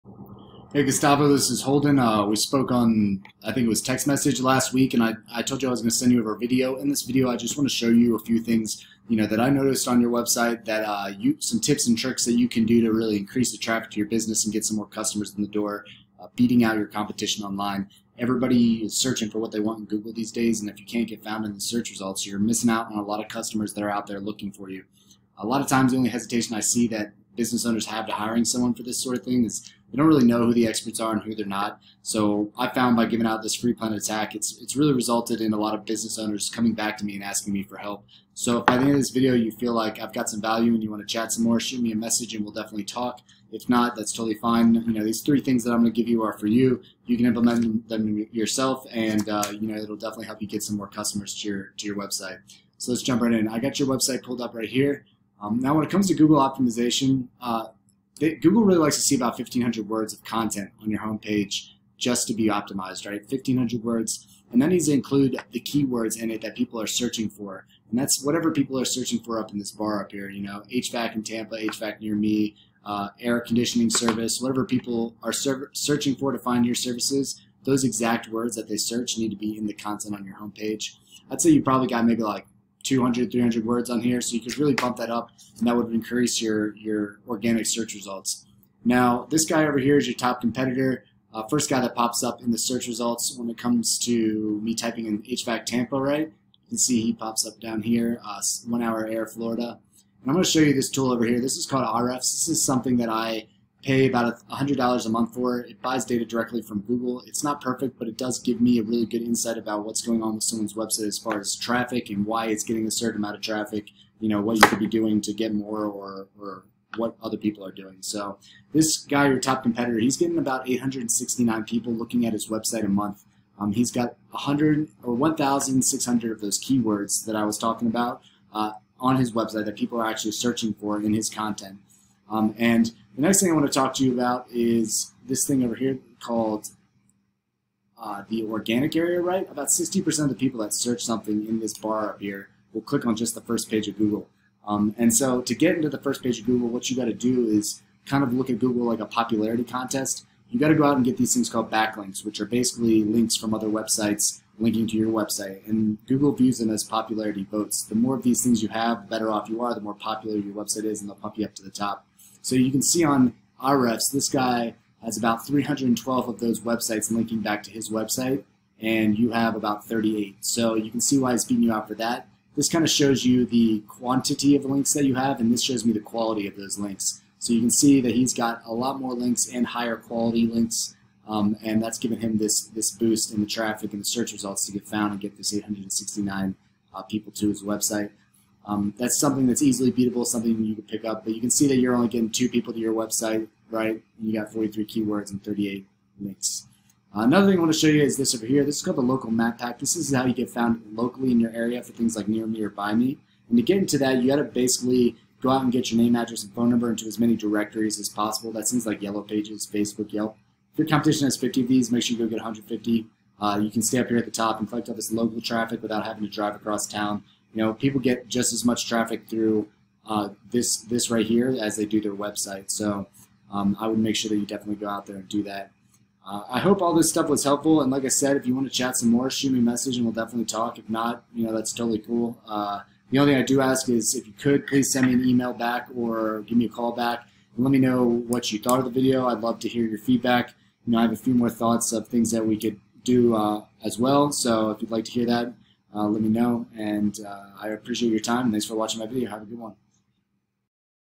Website Analysis for
In this video I show Melo Air
3 tips to increase traffic to their business using the highest ranked  keywords in their area beating out their competition in the online search results.